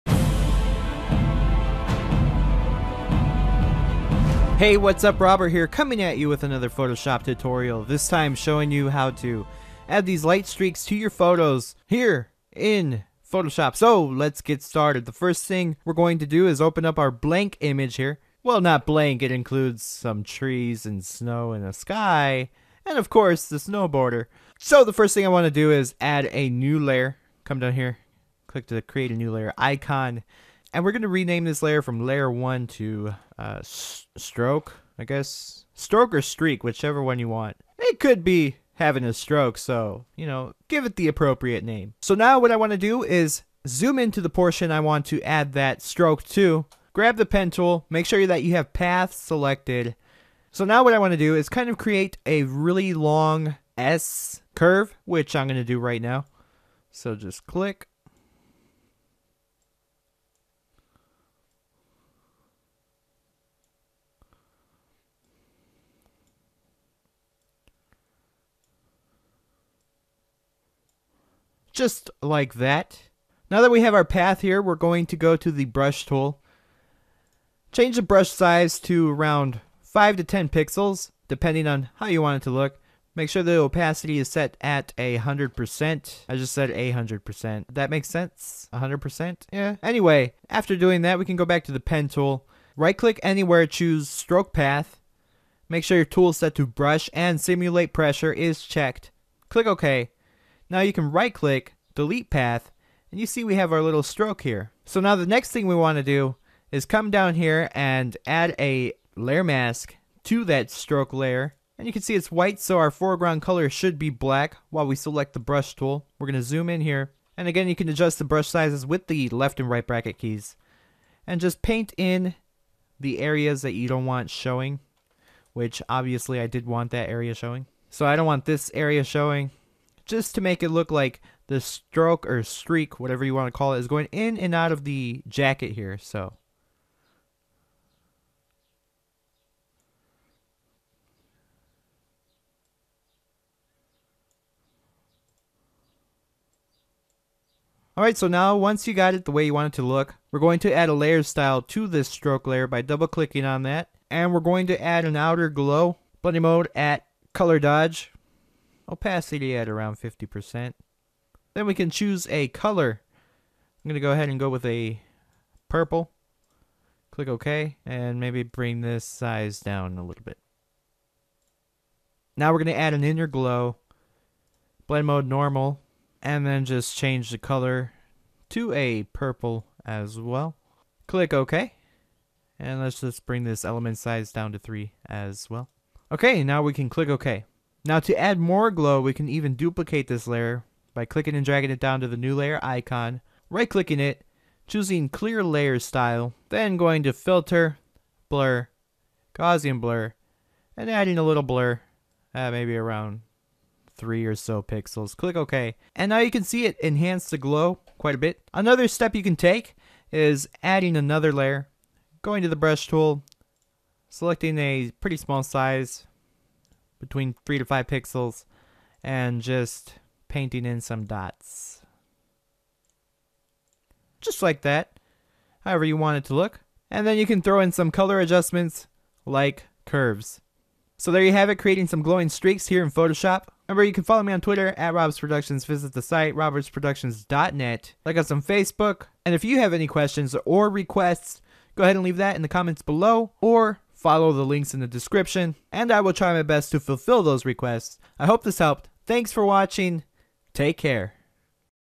Photoshop - Light Streaks
In this tutorial, I show you how to add light streaks to your photos in Photoshop CS5.